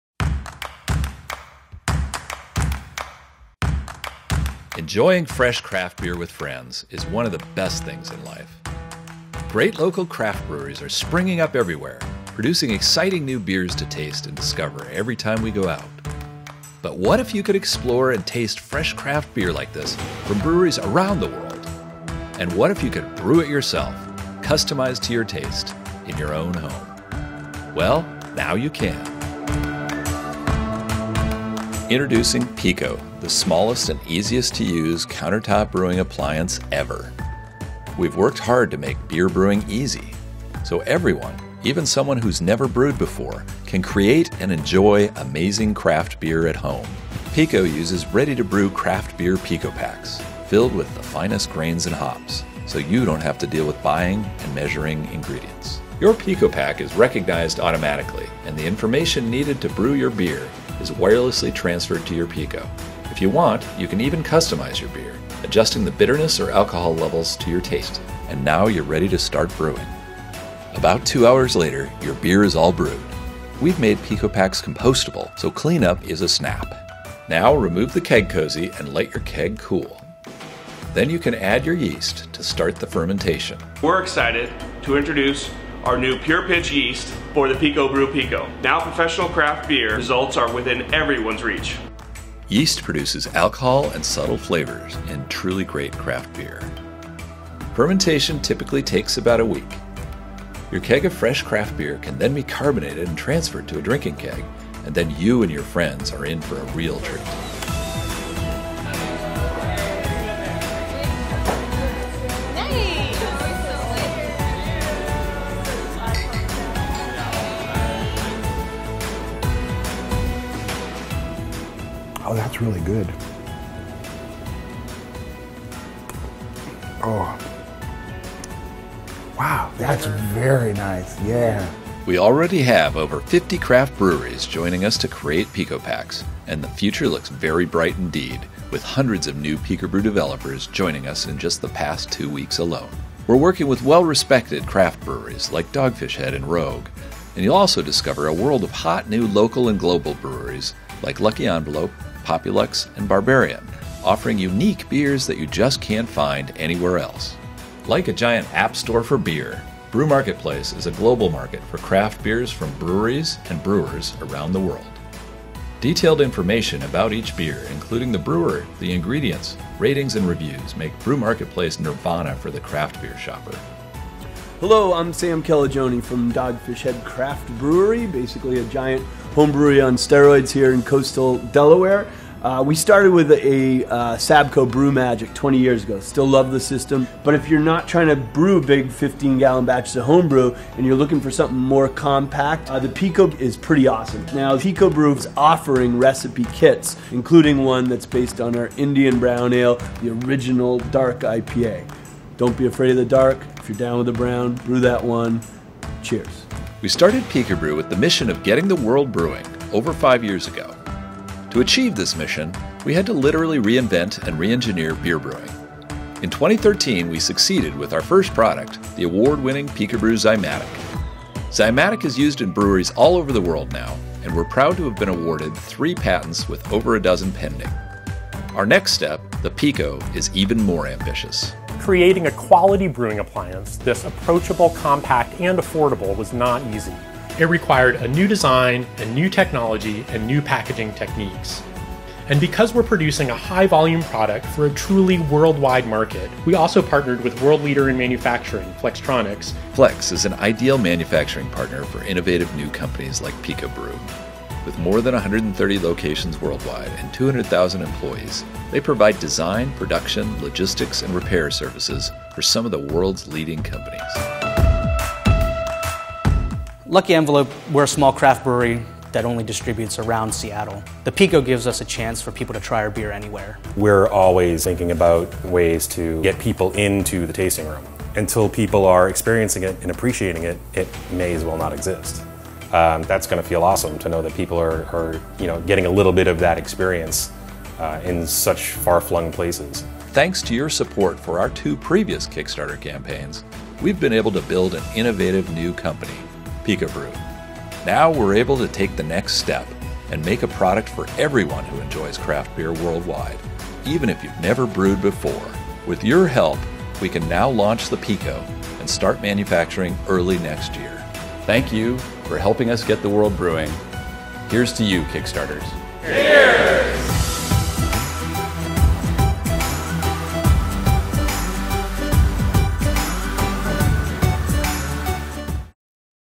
Pico Craft Beer at Home by PicoBrew Inc — Kickstarter
3D-принтер для производства пива в домашних условиях